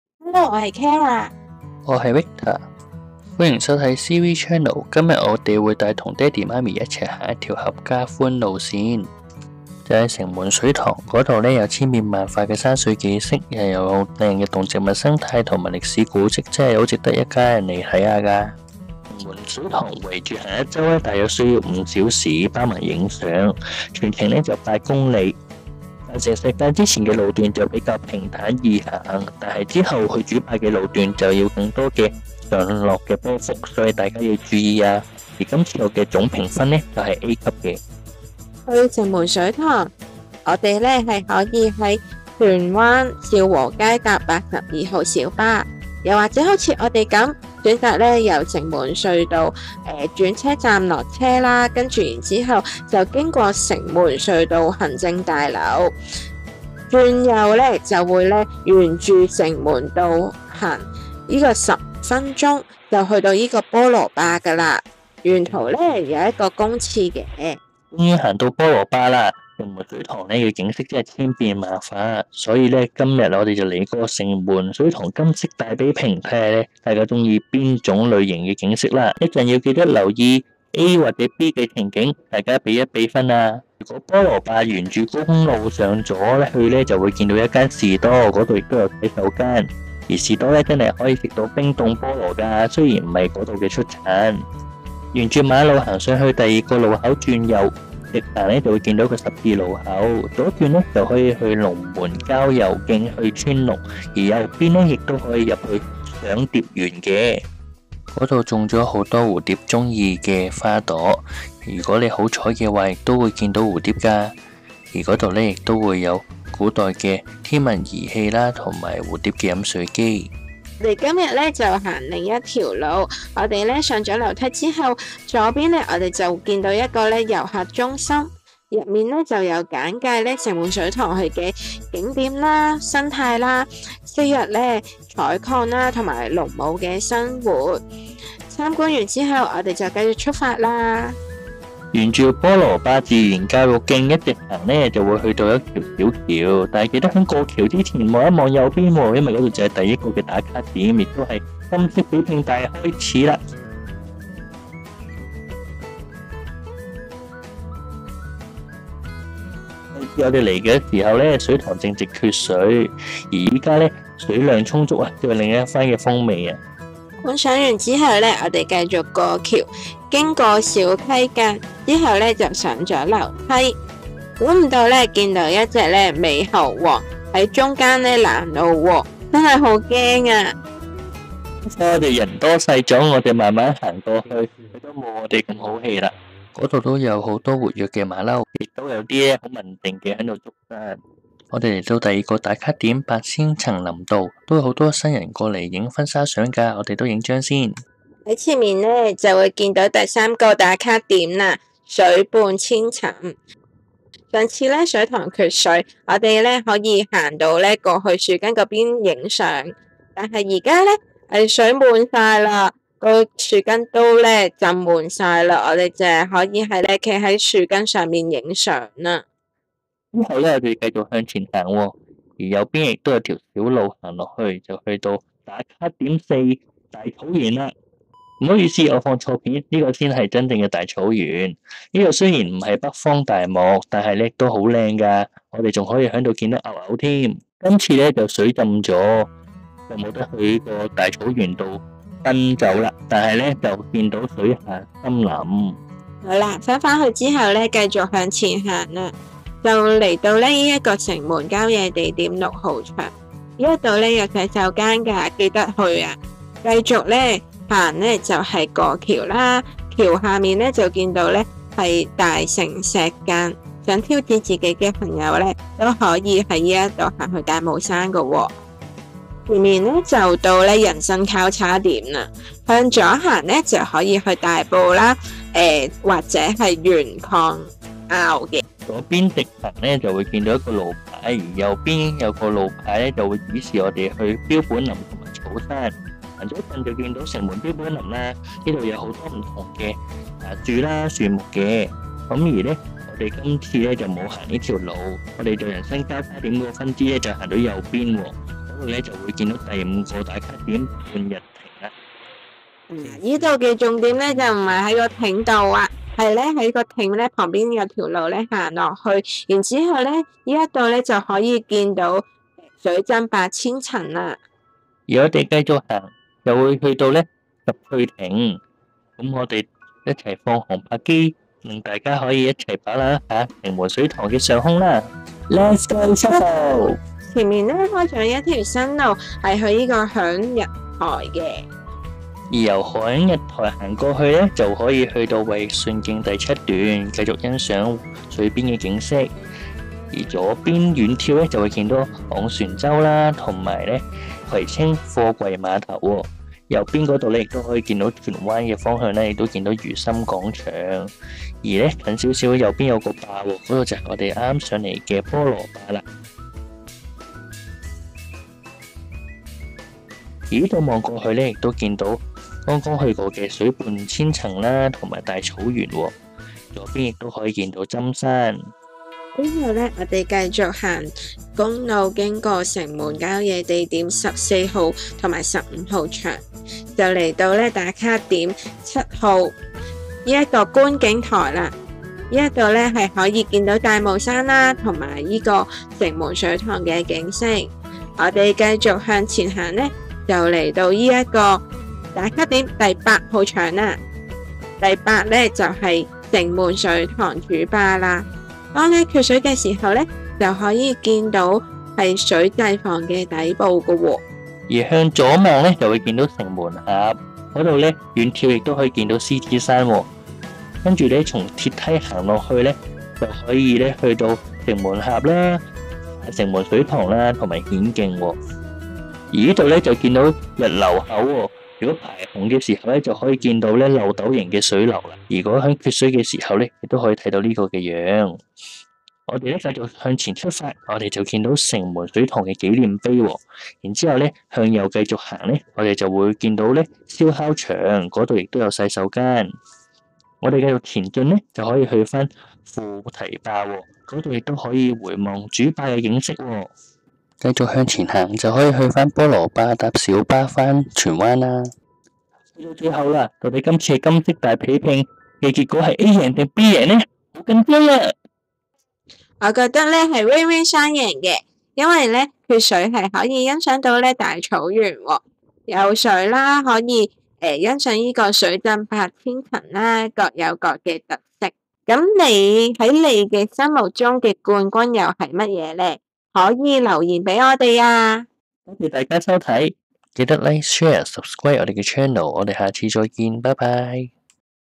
[野外導賞團] 城門水塘乾濕季節兩邊睇
城門水塘
路程:8KM
攀升:303公尺
下坡:141公尺
需時:5小時
路線: 城門隧道-城門隧道行政大樓-城門道-菠蘿壩-賞蝶園/城門郊野公園遊客中心-菠蘿壩自然教育徑-白千層林道-水畔千層-大草原-城門郊野地點6號場-大城石澗-標本林-半閒亭-拾翠亭-觀景台-城門水塘主壩-鐵橋+水掣房-城門水塘紀念碑-城門水塘副壩-菠蘿壩

新界西南部
城門水塘

景觀：👍👍👍👍 不同季節不同景色,有水浸白千層,有大草原,有猴子,有牛,有花草樹蝶,也有歷史古蹟
方便:👍👍👍👍 行去搭車任君選擇
難度:👍👍👍👍 合家歡路線簡單易行,只是後段比較辛苦多上落波幅,但也有新景點值得
評價：A，景色優美，不同時令各有特色,也有教育古蹟,交通方便，CP值極高,一定要去, 但小心馬騮唔好搞佢

0:00 簡介
0:38 到達方法
1:07 賞蝶園
2:01 遊客中心
2:21菠蘿壩自然教育徑
3:11白千層林道-水畔千層
3:42 大草原
4:29 標本林
4:52 半閒亭
5:14 拾翠亭(航拍)
6:08 觀景台
6:31 主壩(航拍)
7:20 副堤壩
7:32 比賽結果